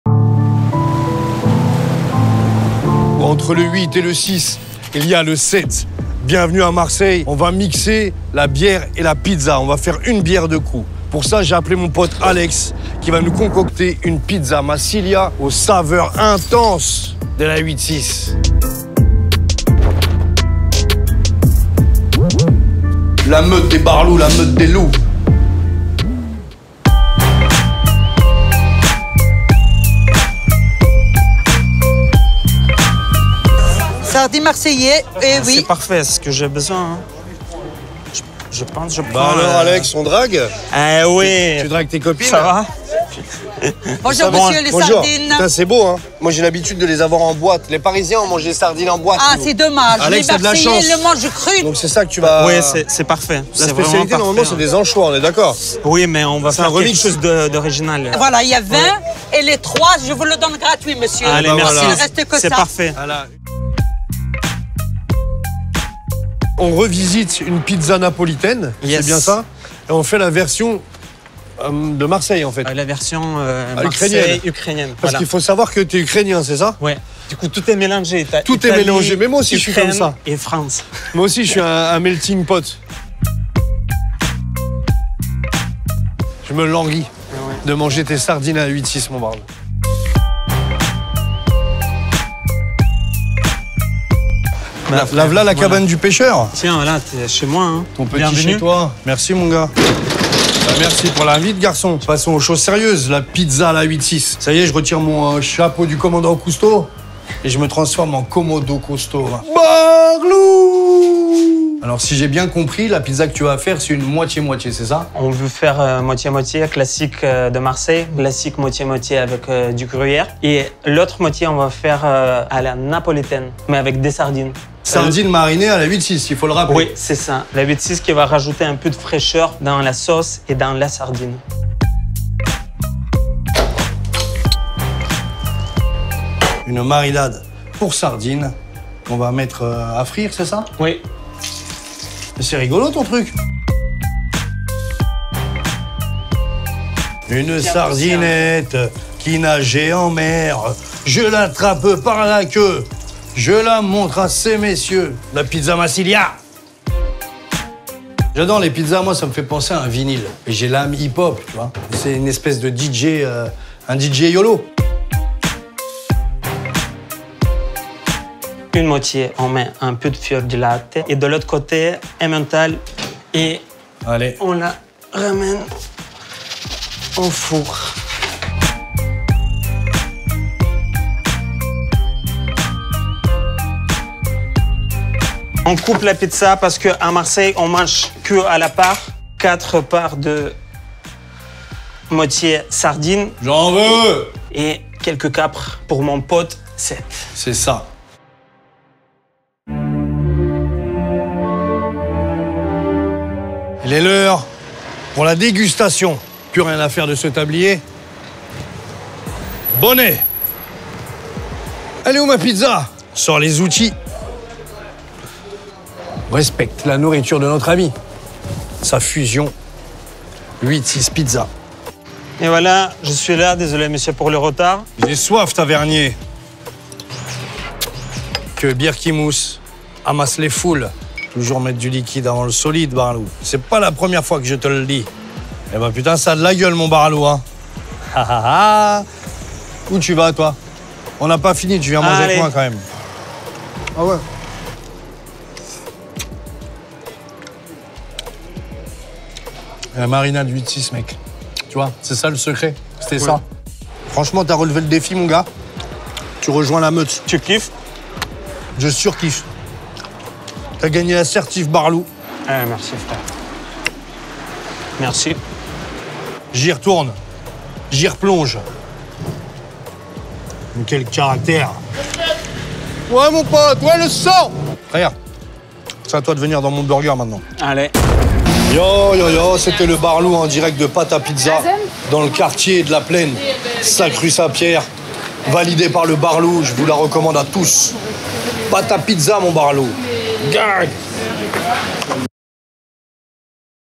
Direction Marseille avec Seth Gueko et 8.6 pour se casser le ventre
La meute des chefs revient et cette fois, direction Marseille avec Seth Gueko et Alex Sullivan pour concocter un maxi food pairing pizza et 8.6 🔥 Attention, recette intense et de caractère💥
En collab avec Vice France.